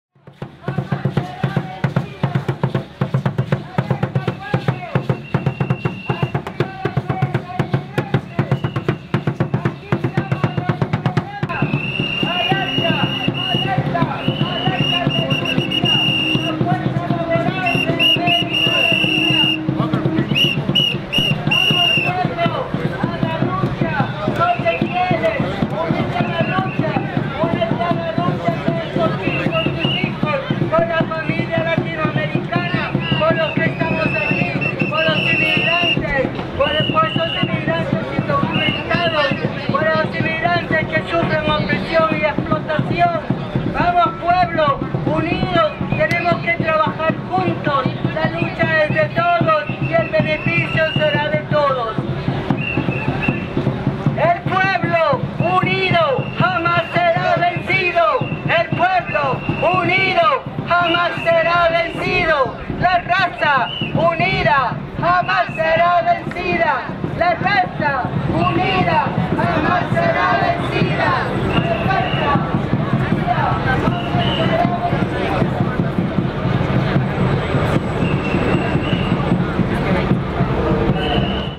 May Day 2014
May Day march 2014, leaving 24th & Mission BART station.